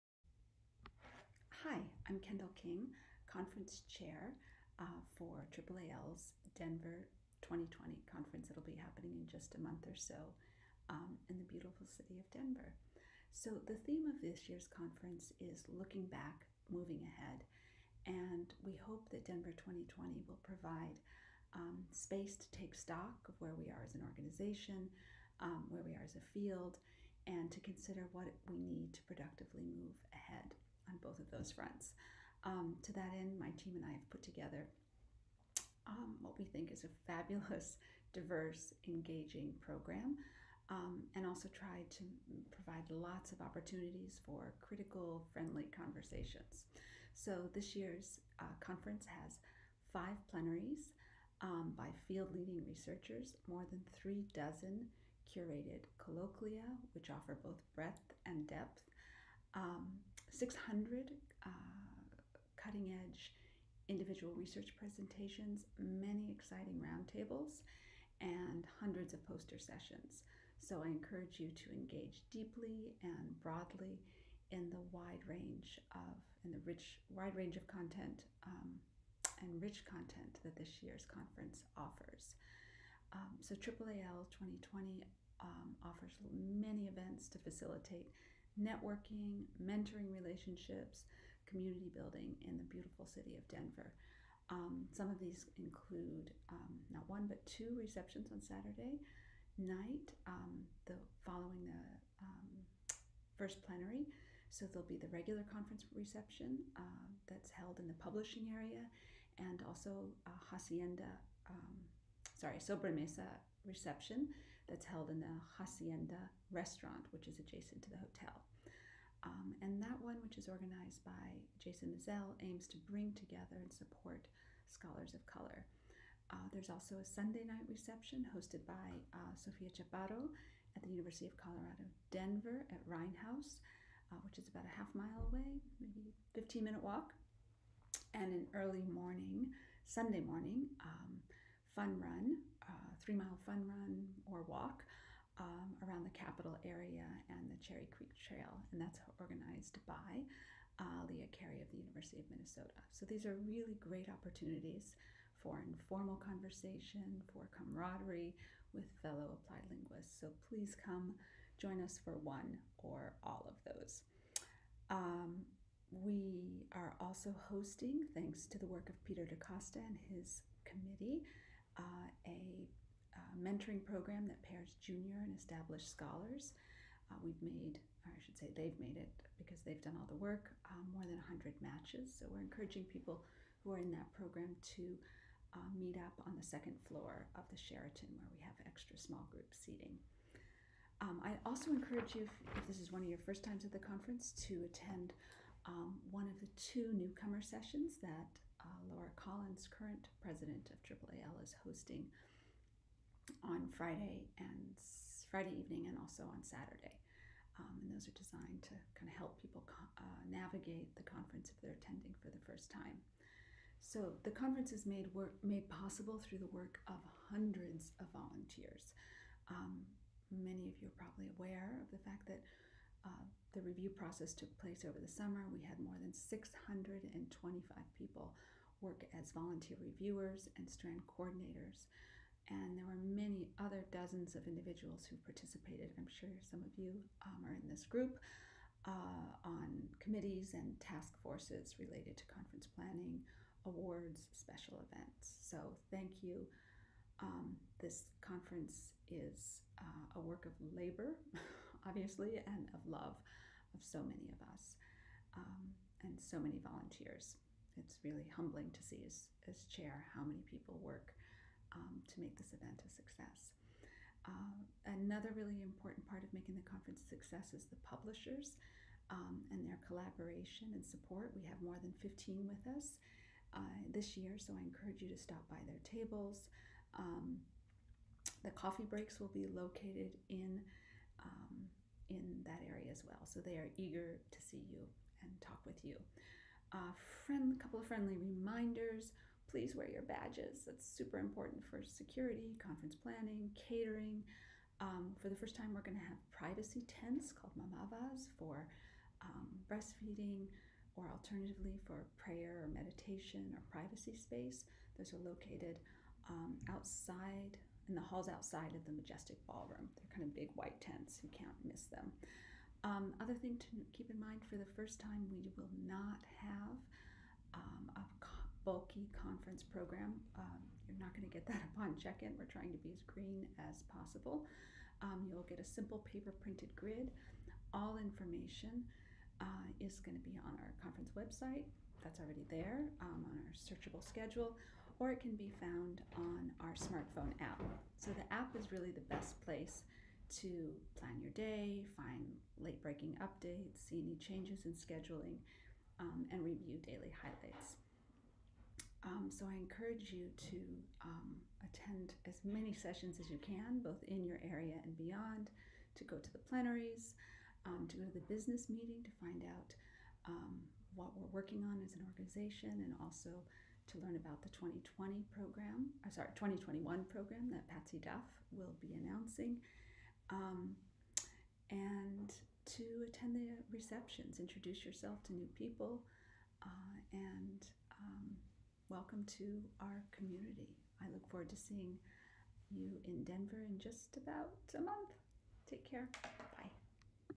Conference Chair Dr. Kendall King on 2020 AAAL Conference
In this video, Dr. Kendall King, Professor at the University of Minnesota and 2020 AAAL Conference Chair talks about the plans and preparations for the Denver conference.